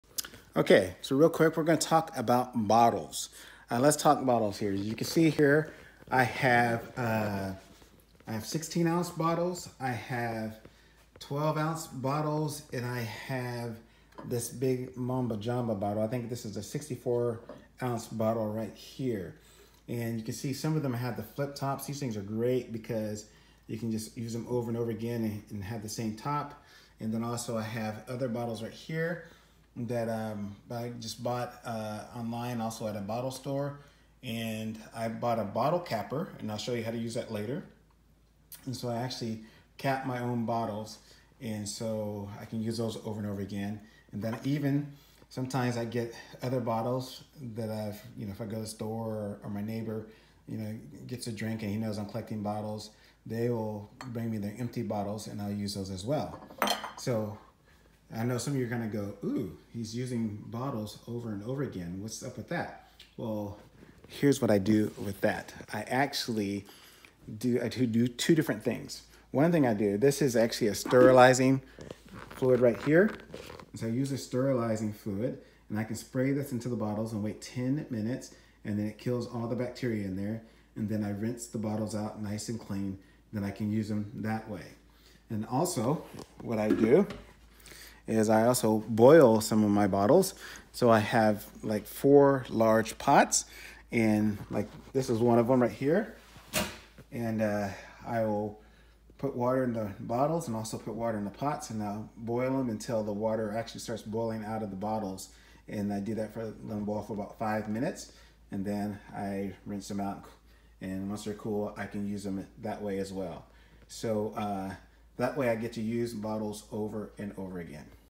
Options for bottling your kombucha.
It’s important to use bottles that are designed to handled pressure.